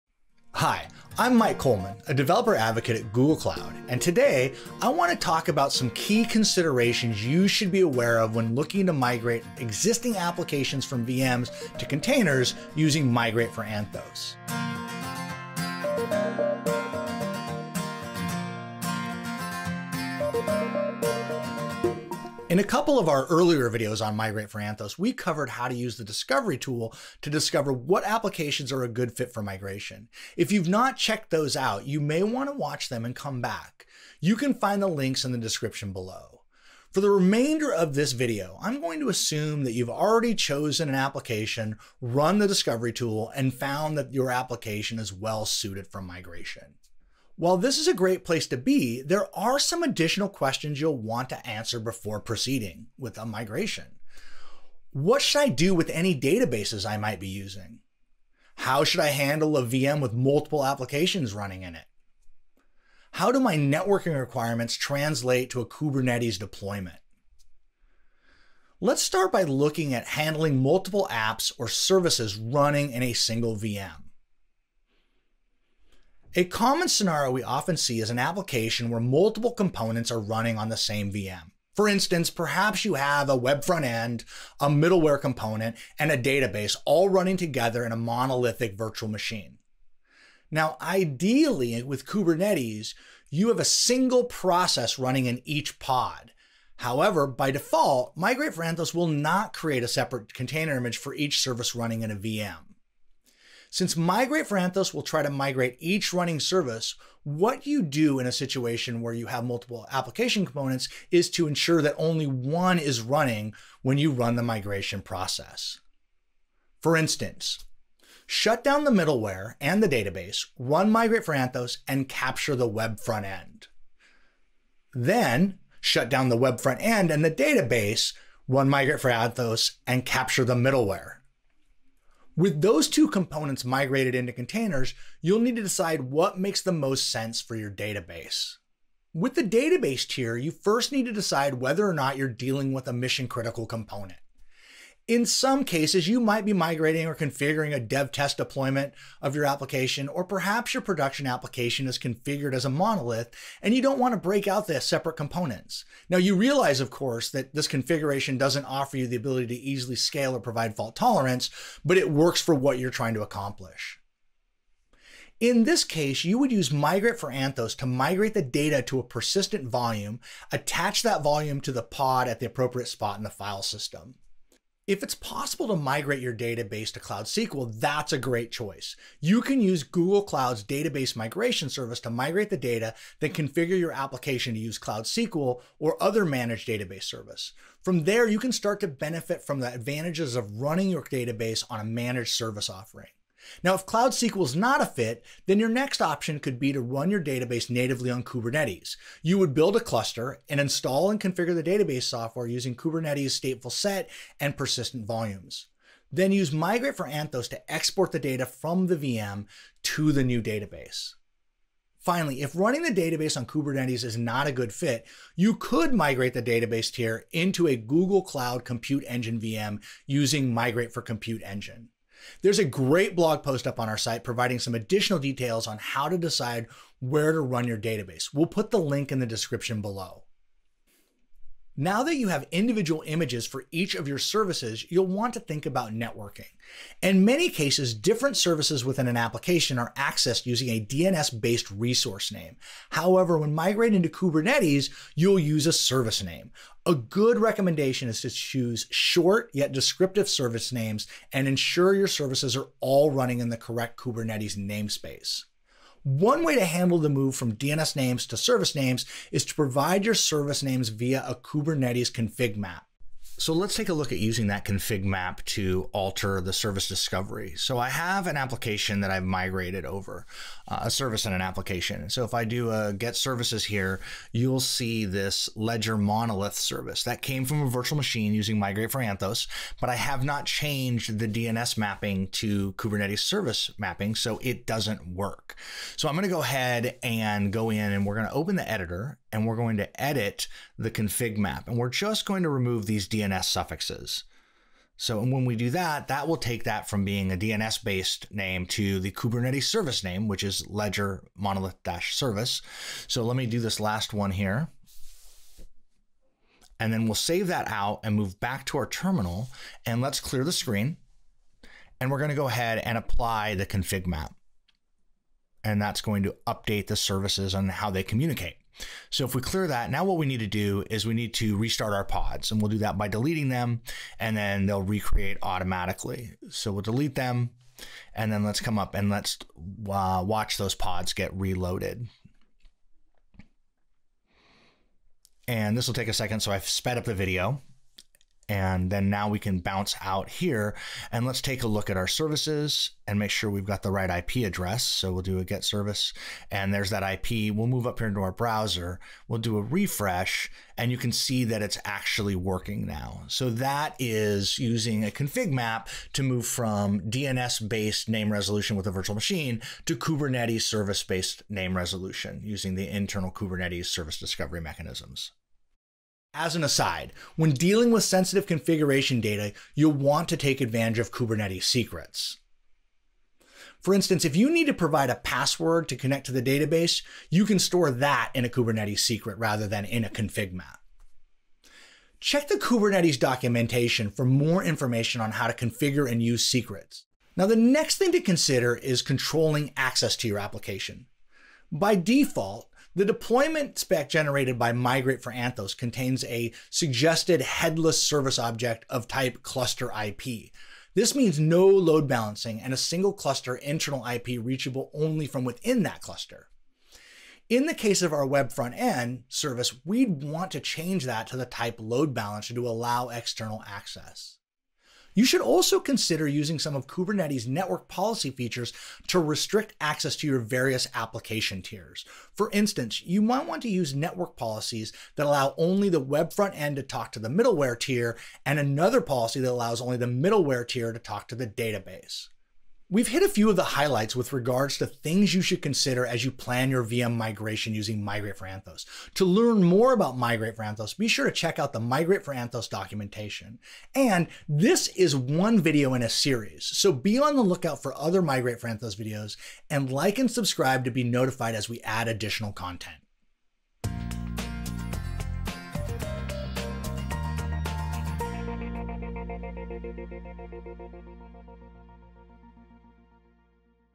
Assessing applications for migration
This video addresses what key issues developers should be aware of when looking to migrate existing applications from VMs to containers using Migrate for Anthos. To learn more about Migrate for Anthos be sure to check out the Migrate for Anthos documentation.

Chapters:

0:00 - Intro
1:17 - Migrating multiple apps in a single VM
2:18 - Choosing a database
4:20 - Providing service names via Kubernetes config map
6:28 - Controlling access to your application
7:13 - Recap

Product: Migrate for Anthos;  fullname: Mike Coleman;